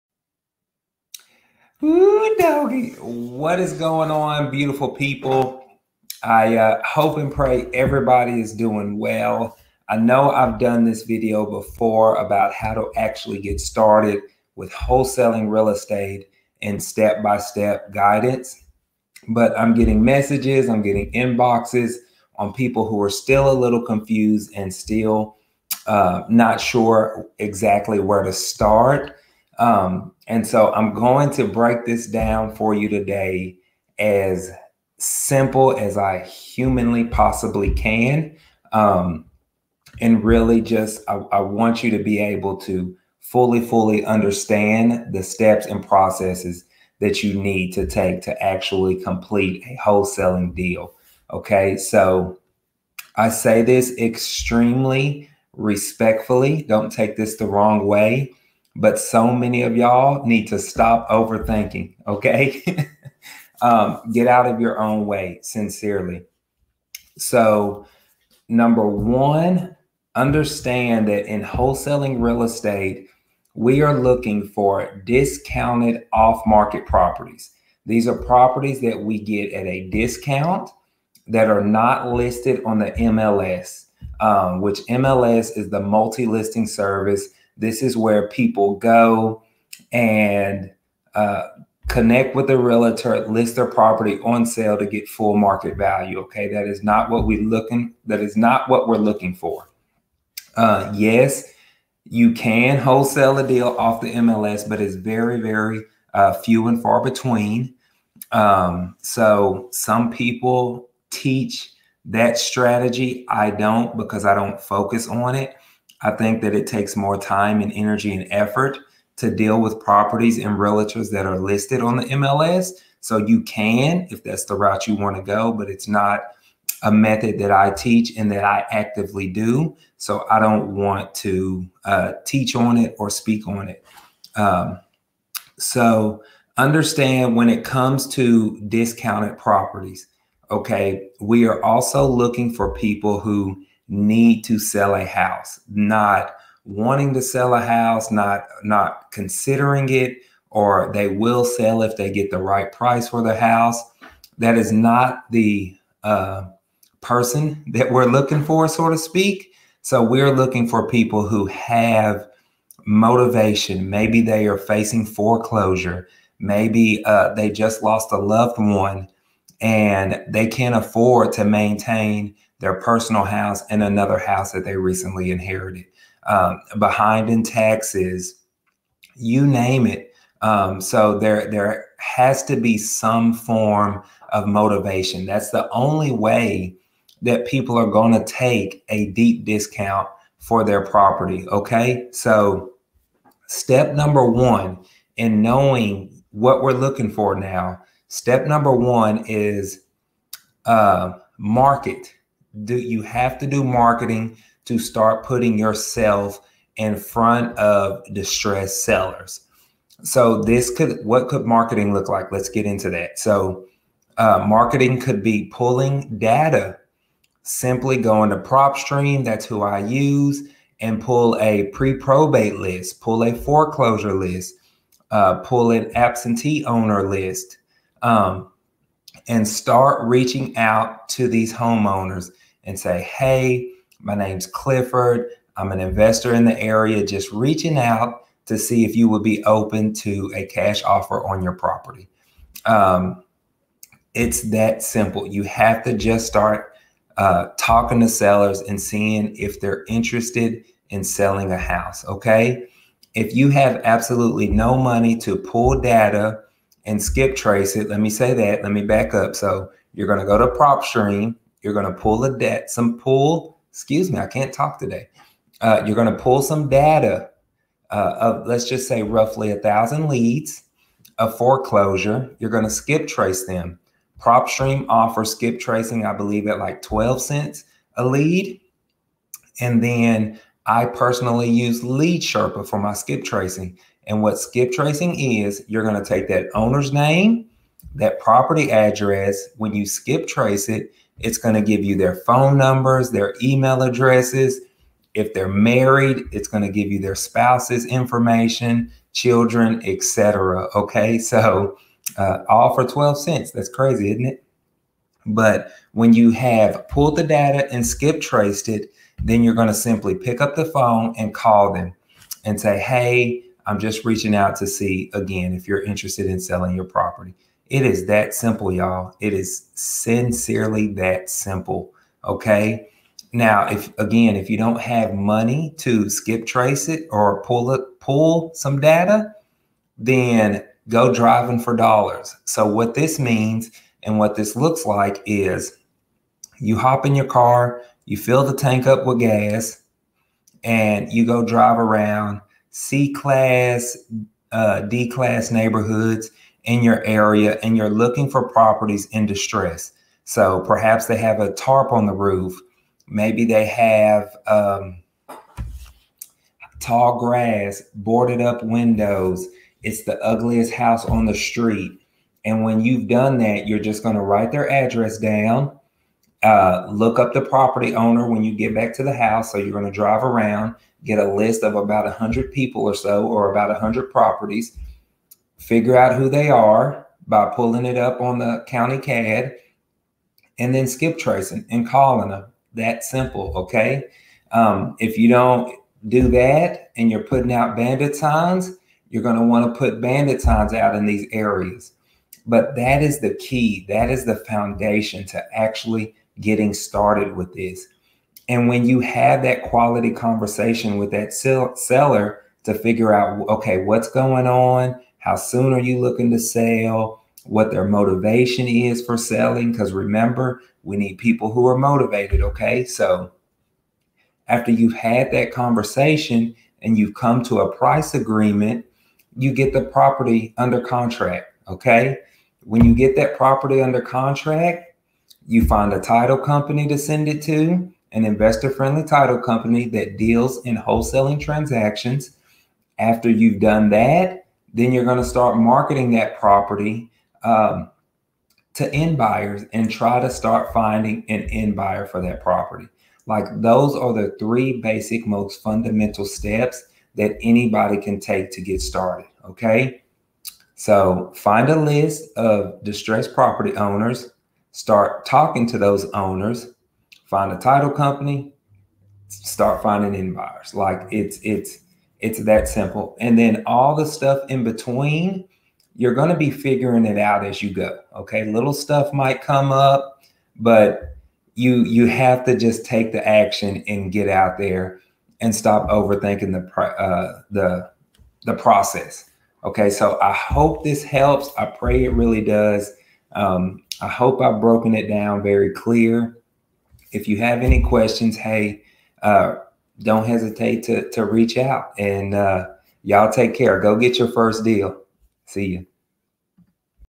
Wholesaling Houses For Beginners - How To Get Started
Here are a few basic steps for anyone to use to get started wholesaling.
Please don't overthink it.